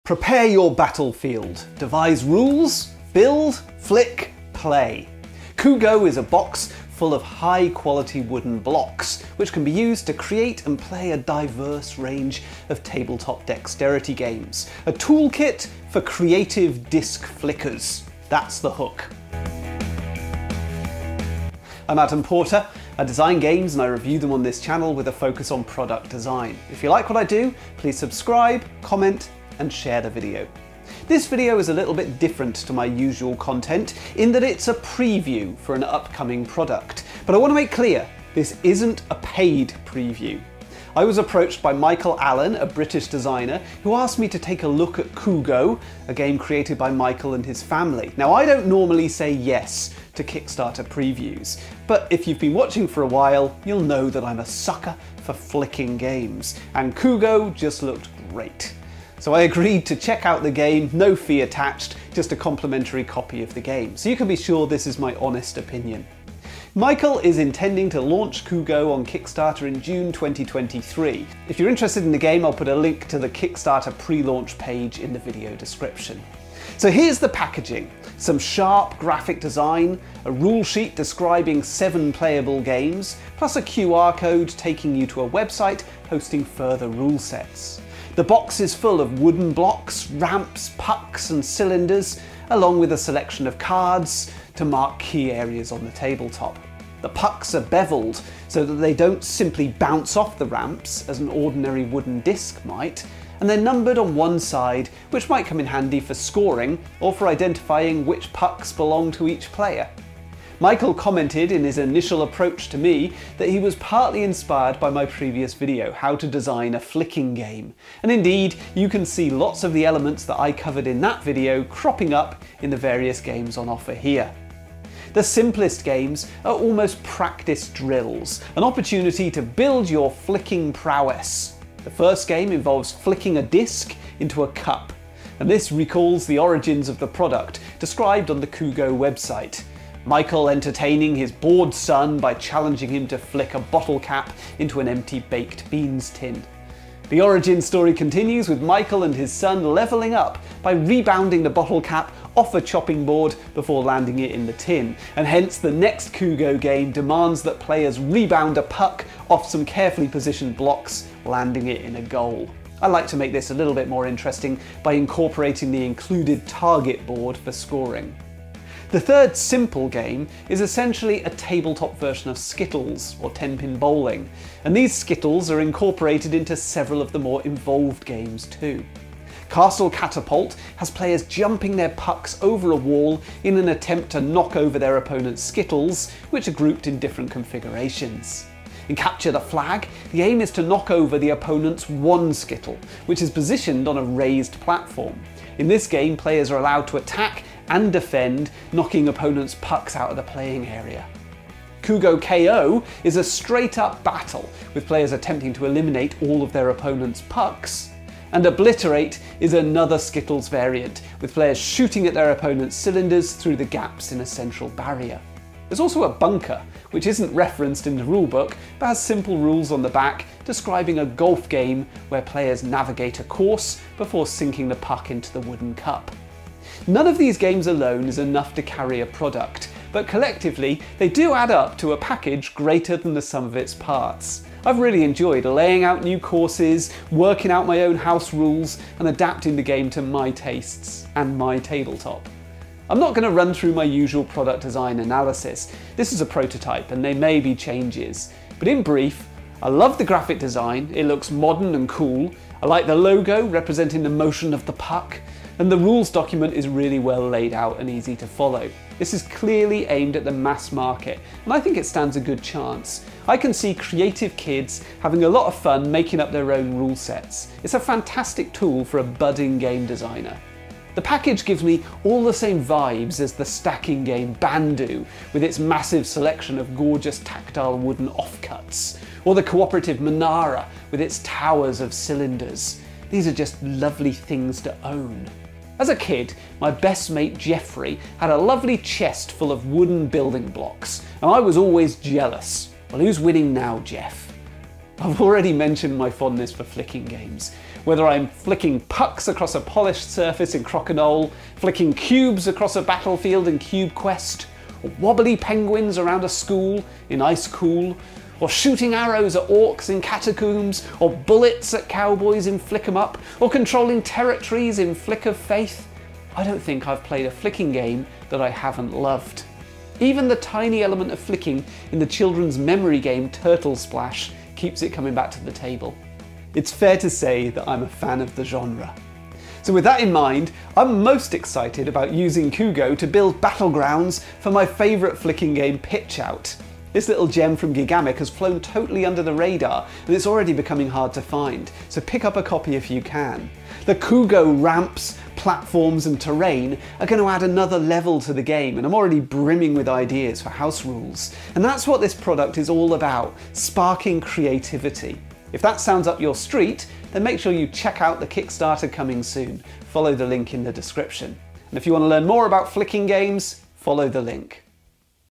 Build, Flick, Play: KUGO - a toolkit for creative disc flickers.
Prepare your battlefield; devise rules; build, flick, play. KUGO is a box of high quality wooden blocks which can be used to create and play a diverse range of tabletop dexterity games. A toolkit for creative disc flickers. That’s the hook.

This video is a little different to my usual content, in that it’s a preview for an upcoming product.

I was approached by Michael Allan, a British designer, who asked me to take a look at KUGO - a game created by Michael and his family. I don’t normally say Yes to Kickstarter previews. But if you’ve been watching for a while, you’ll know I’m a sucker for flicking games, and KUGO just looked great.

So I agreed to check out the game; no fee attached - just a complimentary copy of the game - so you can be sure this is my honest opinion.

Michael is intending to launch KUGO on Kickstarter in June 2023. If you’re interested in the game, follow the link: